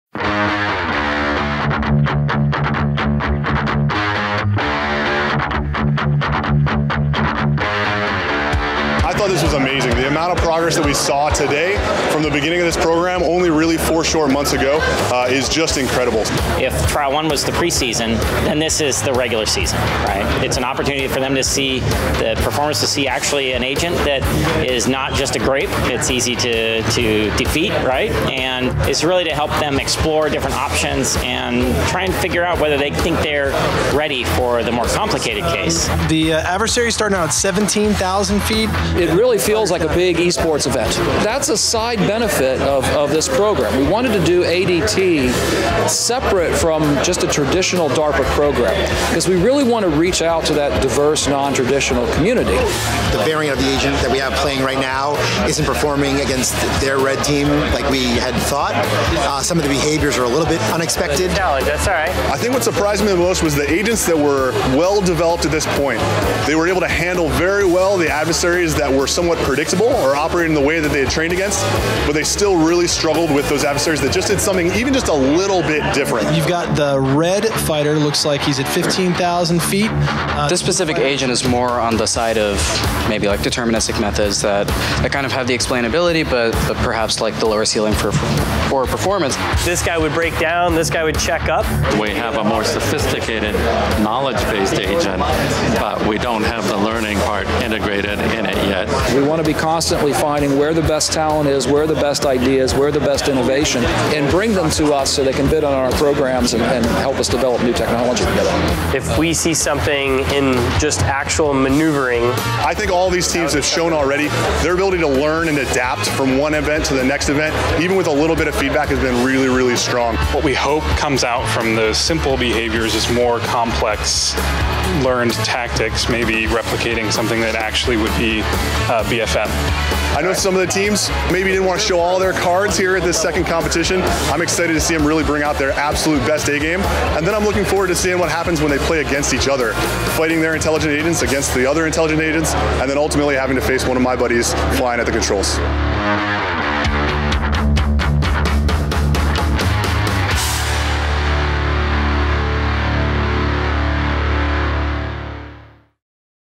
Blue Against Red: AlphaDogfight Trials at APL
*Video originally shot in January of 2020*

Johns Hopkins APL plays a critical role in developing and managing the simulation environment and adversary agents for DARPA's AlphaDogfight Trials, in which teams pit their "blue" AIs against APL's "red" AIs. The Lab hosted the first AlphaDogfight Trials competition event in November of 2019, and hosted the second one in the Intelligent Systems Center in January of 2020.

The DARPA AlphaDogfight Trials seeks to advance the state of artificial intelligence technologies applied to air-combat operations. The computer-based competition is designed to demonstrate advanced AI algorithms that can perform simulated, within-visual-range air combat maneuvering, otherwise known as a dogfight. DARPA selected eight technically and organizationally diverse teams to compete in the AlphaDogfight Trials, looking to energize and expand a base of researchers and developers applying AI technologies to complex operational problems.

"Ultimately, with ACE, we will be flying these new algorithms on real live aircraft as we progress toward deployable autonomous air combat systems," said APL's Matt Rich, principal investigator for the trials.

The work is an opportunity to demonstrate APL's technologies and expertise to potential Department of Defense users. The project is also a great example of collaboration across APL sectors and departments.